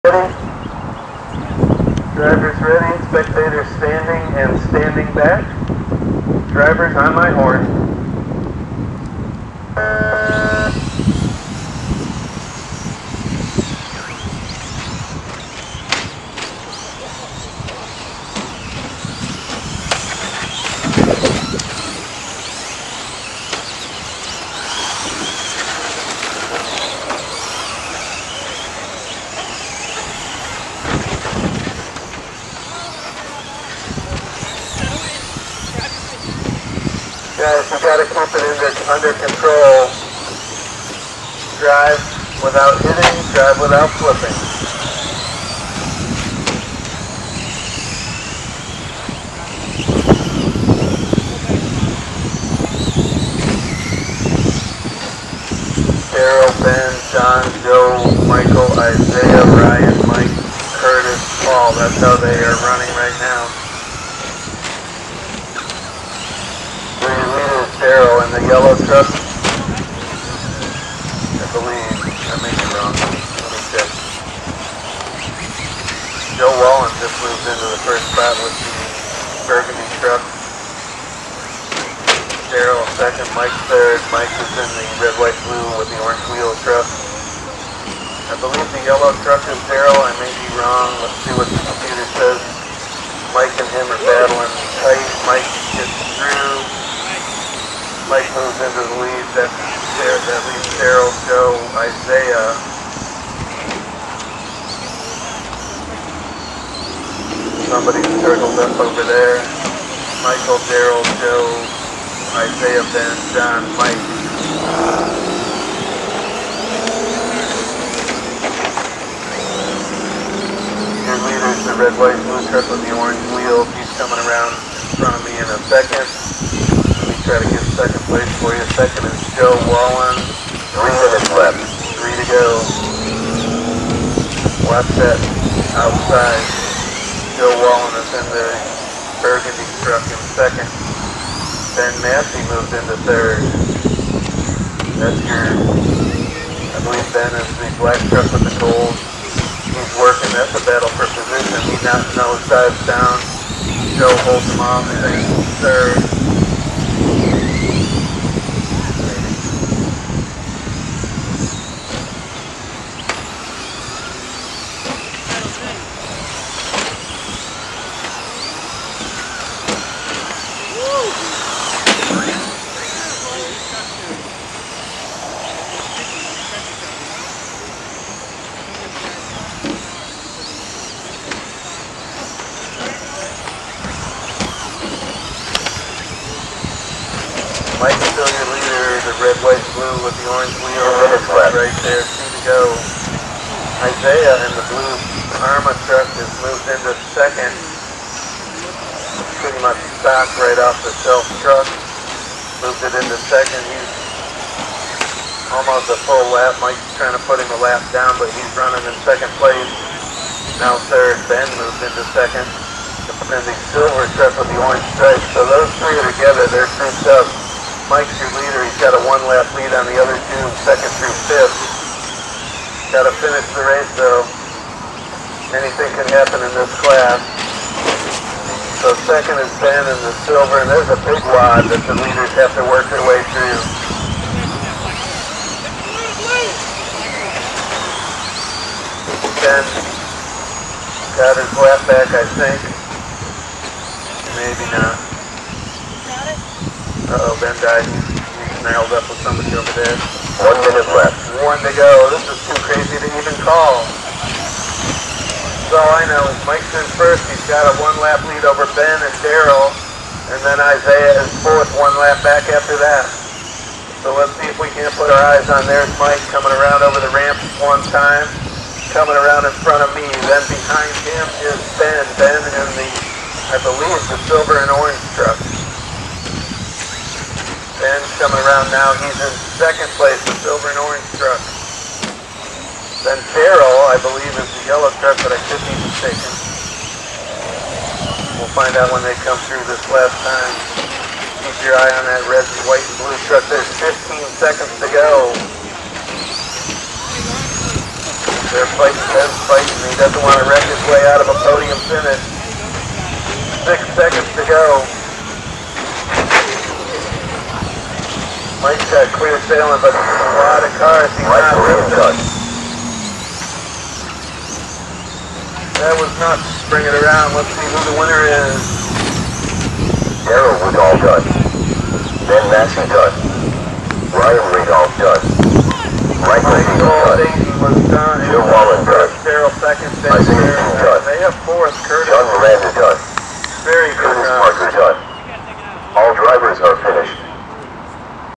2WD Short Course A-Main - June 2nd, 2024 Parking Lot Race
RC Parking Lot Racing
Sunday, June 2nd, 2024
HobbyTown Richmond - 9900 West Broad St. Glen Allen, VA 23060